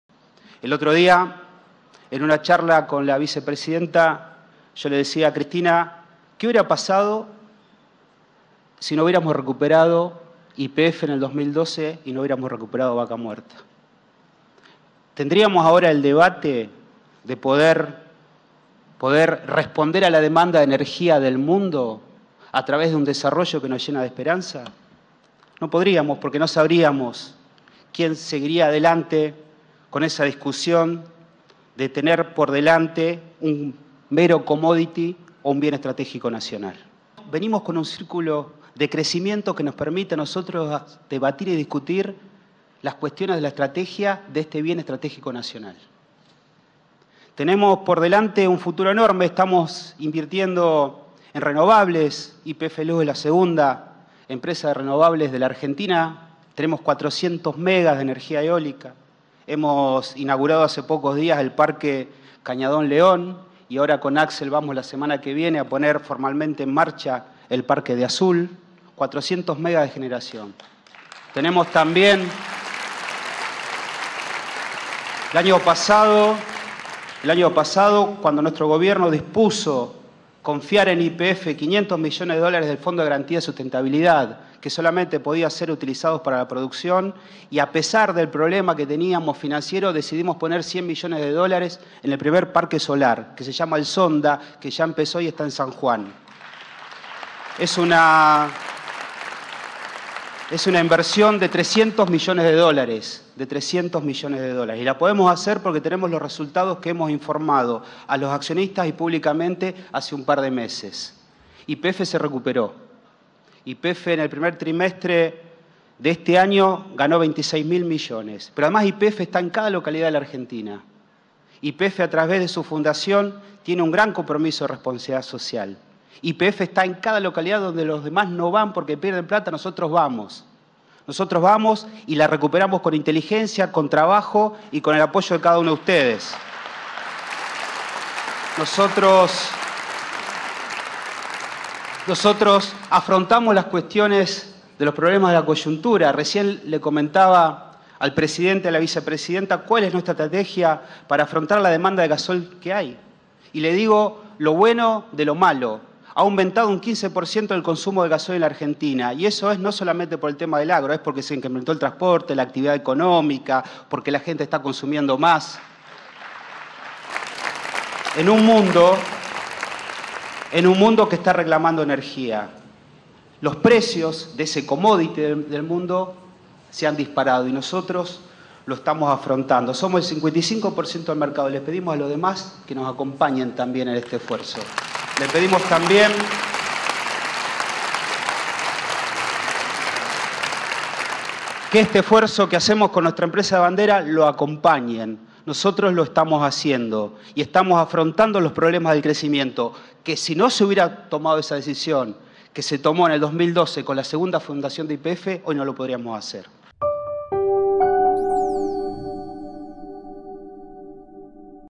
Pablo González: "Con YPF estamos afrontando los problemas de crecimiento"
El presidente de YPF, Pablo González, afirmó hoy que la recuperación de la compañía en 2012 permite hoy afrontar los problemas de crecimiento y la mayor demanda energética, en un contexto de volatilidad global, al participar del acto por los 100 años de la petrolera que encabezaron el presidente Alberto Fernández y la vicepresidenta Cristina Kirchner en Tecnópolis.